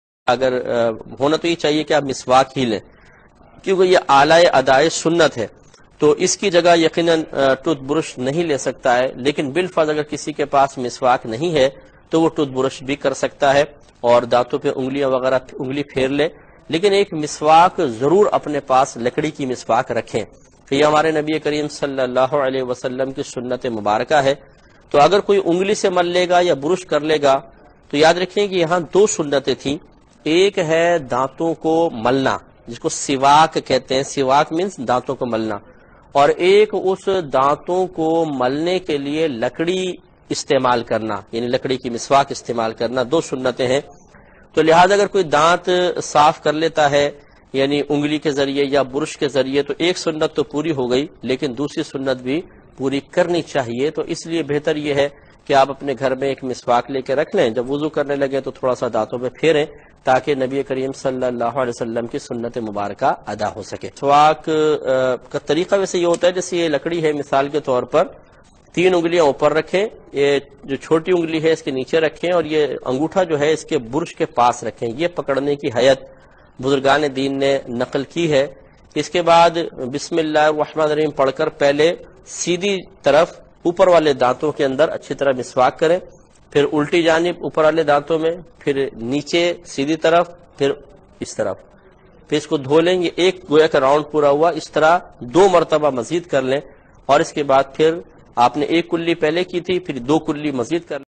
Miswak Karne Ka Sunnat Tariqa aur Masa'il – Kta Brush se Miswak Karsakte Hai ? By Mufti Akmal Sahab
Miswak Karne Ka Sunnat Tariqa aur Masa'il – Kta Brush se Miswak Karsakte Hai ?

By Mufti Muhammad Akmal Qadiri Sahab, Pakistan

Must Listen and Share it